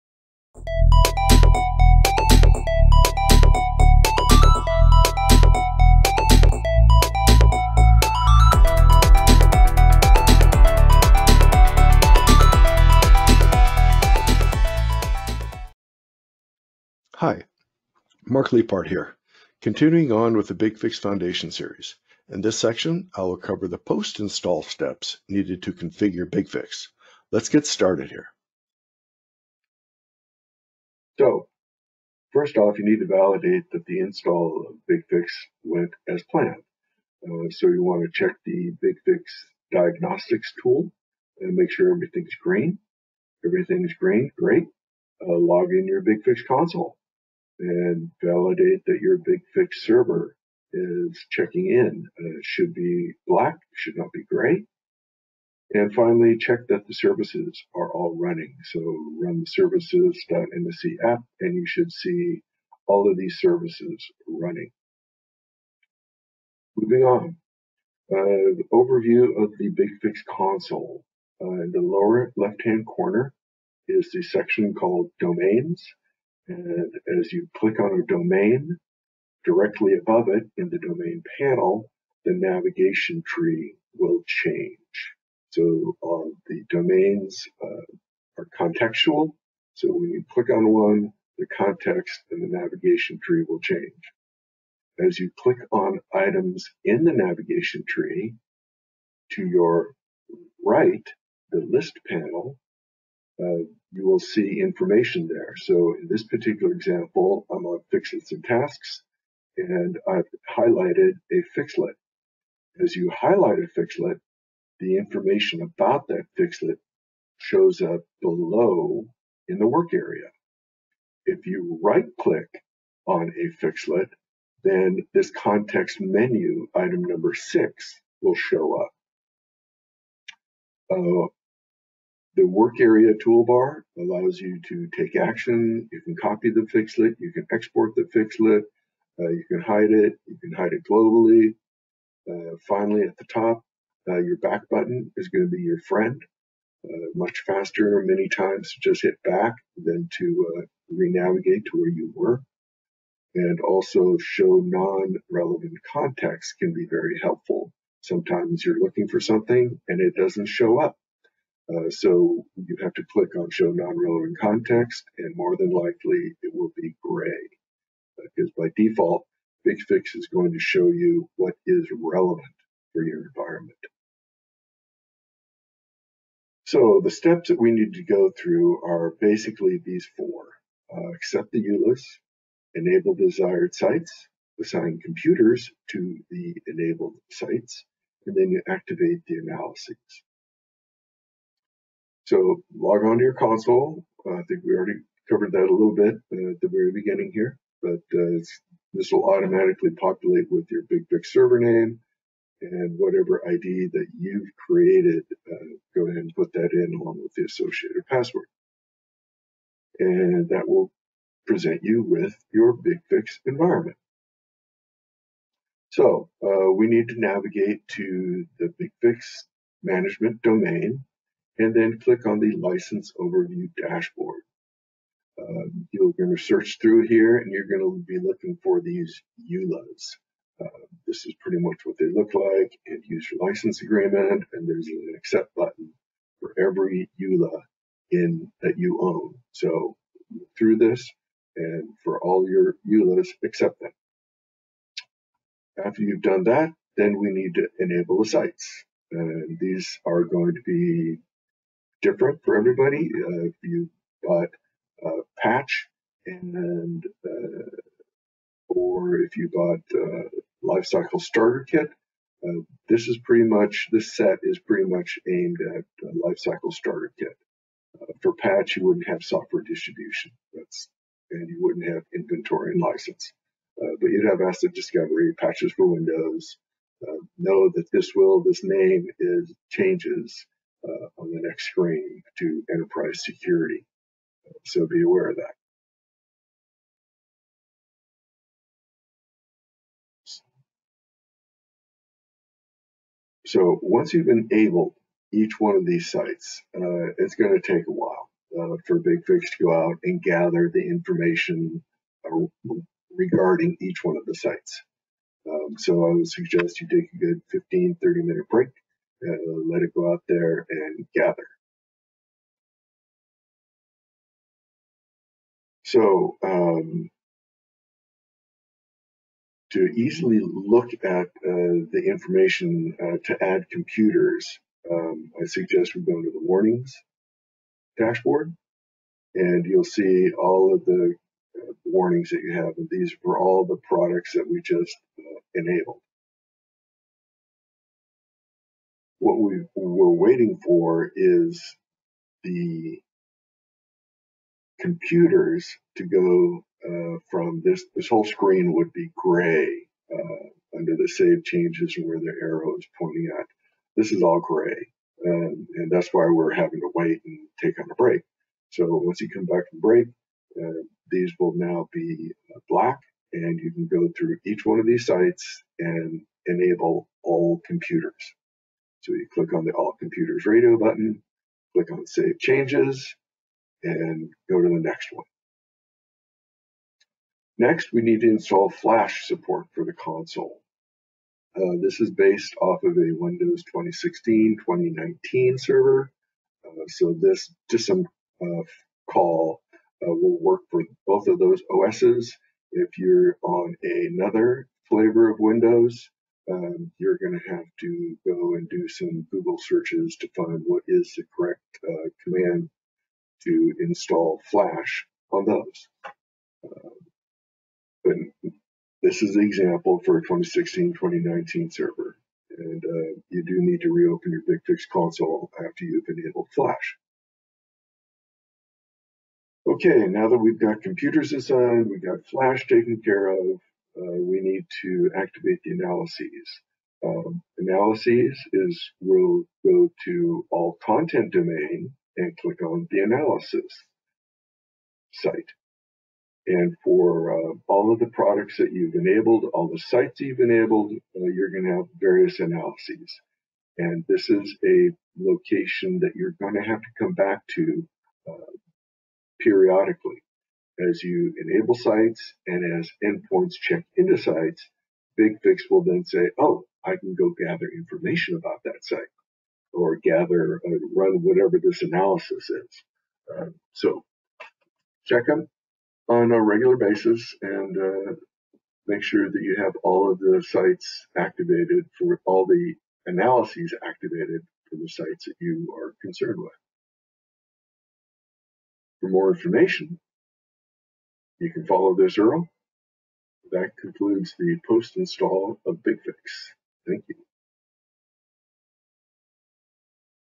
BigFix Foundation Series - Post Install
In this special series created by our Director of BigFix Enablement.  A variety of topics are covered to help a new user better understand BigFix.

This video discusses concepts and topics to consider right after the BigFix Server is installed.